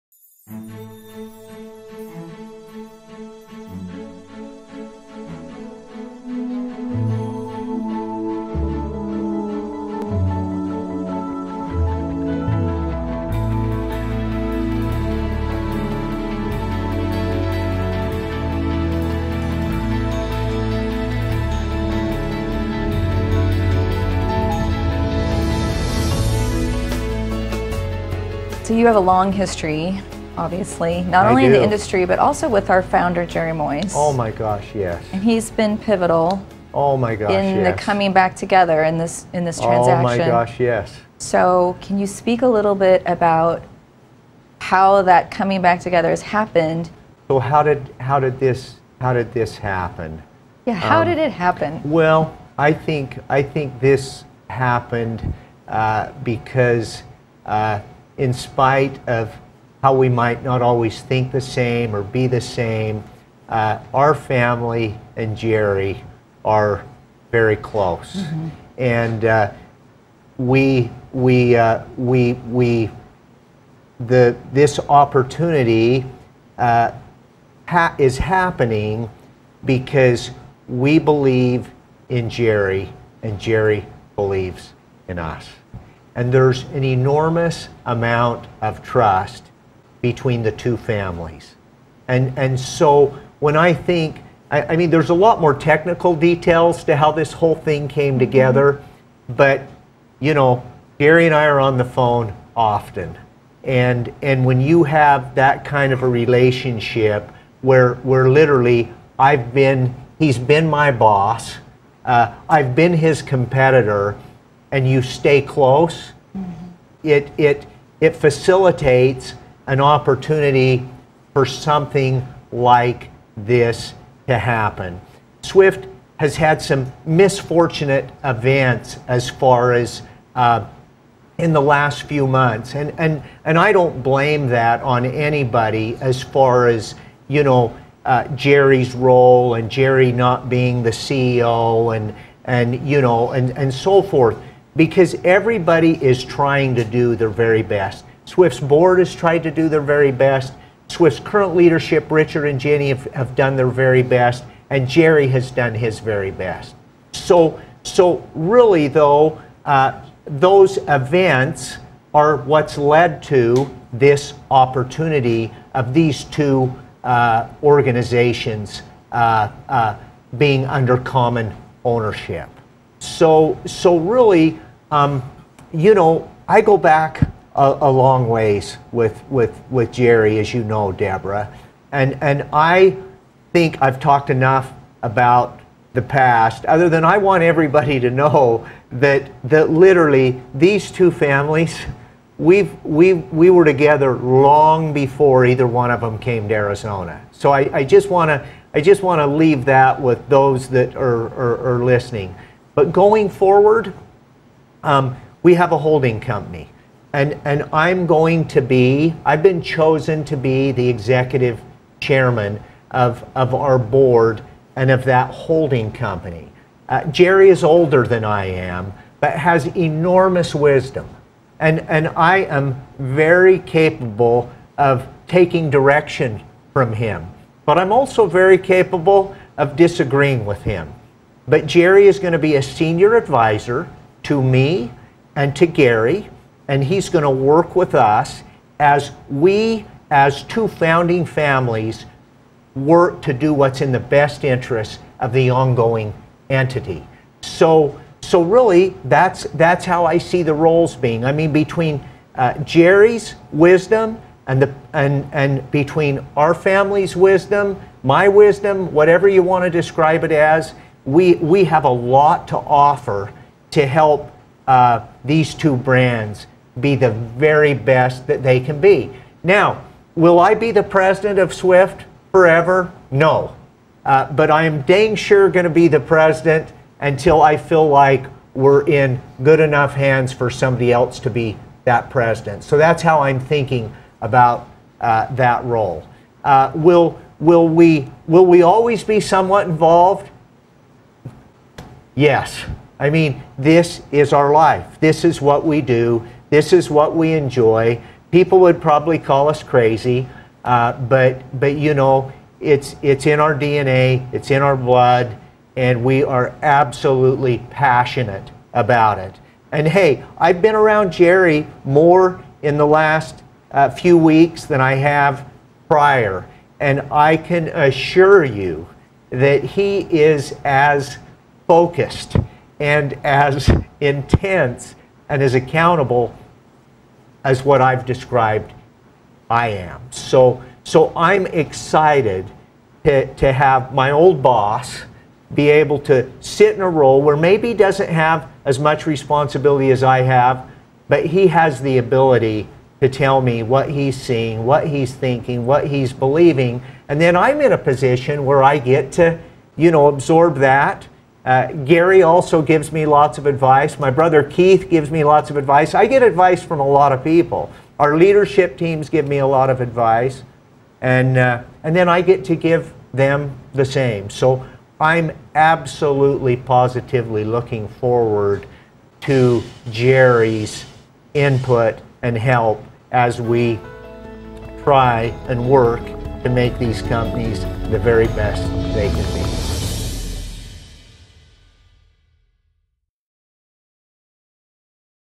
Wisdom (2017)
In this 8-part series Kevin Knight discusses the corporate merger that sent shock waves in the trucking industry as SWIFT transportation joins KNIGHT transportation